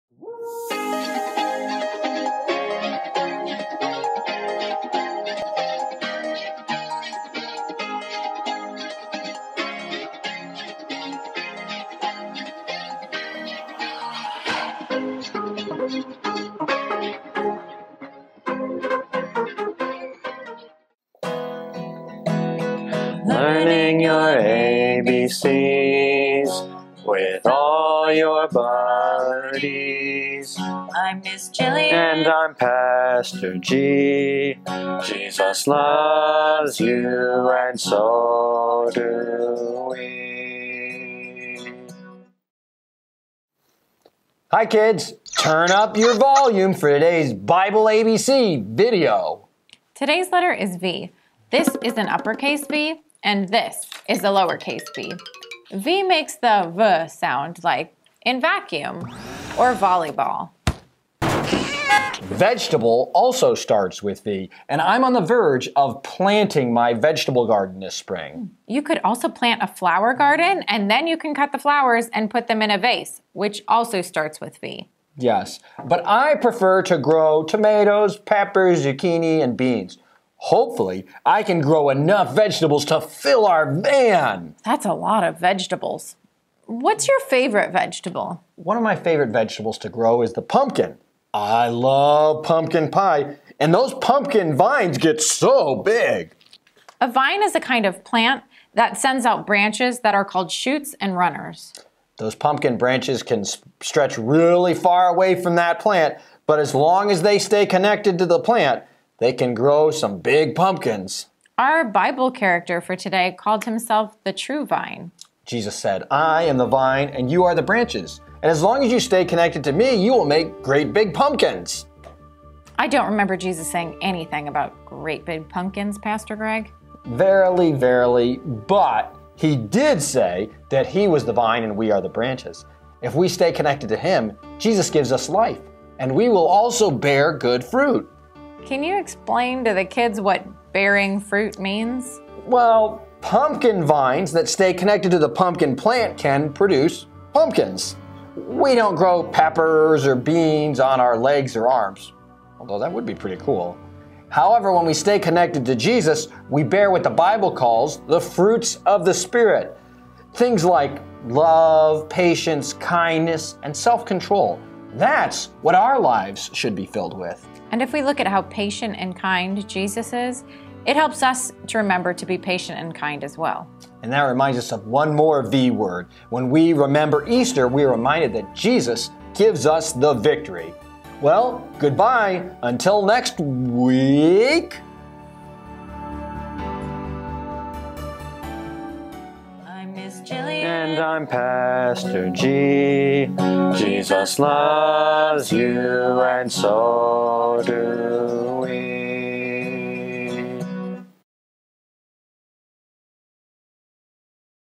"V" | Bible ABCs
Voila! Here is another very vivacious version of Bible ABCs coming your way! Not only is Jesus the True Vine but also gives us the true Victory!

And remember, LIVE in God's grace and never GIVE up hope!

Intro Animation Credit:
Nathan Behmlander